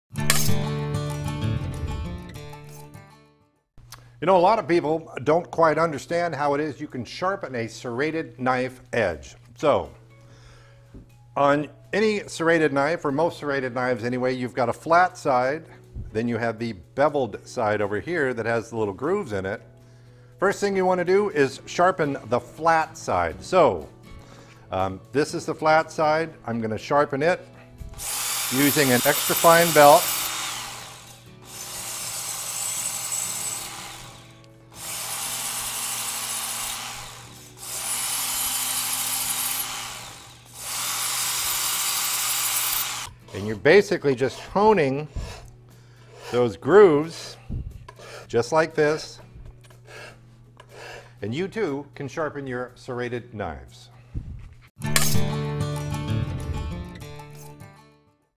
SHARPEN SERRATED KNIVES
How to sharpen serrated knives. Chef Scott shows how to sharpen serrated knives using the Work Sharp Ken Onion Knife and Tool  and Guided Field Sharpeners.